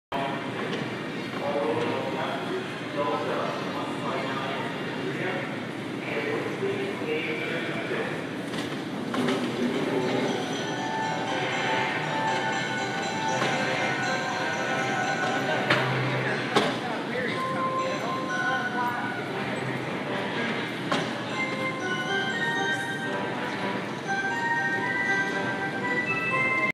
Detroit airport's musical corridor
In the Detroit airport, the really long trip through the corridor between concourse A and B is made less painful by the fun light show and music :)